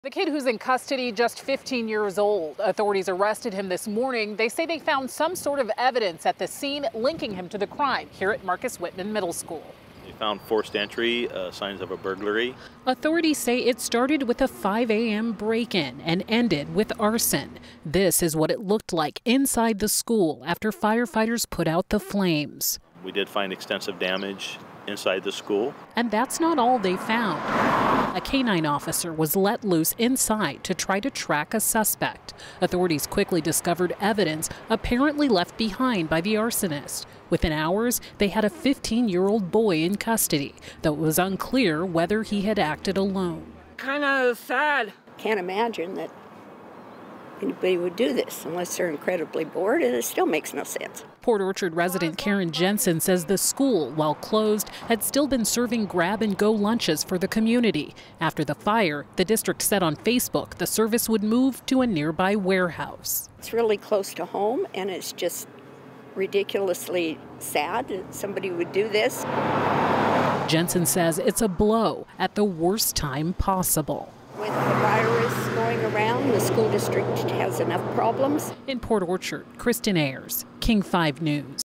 Fire set inside middle school in Port Orchard
A 15 year old suspect is being questioned in the burglary at Marcus Whitman Middle School.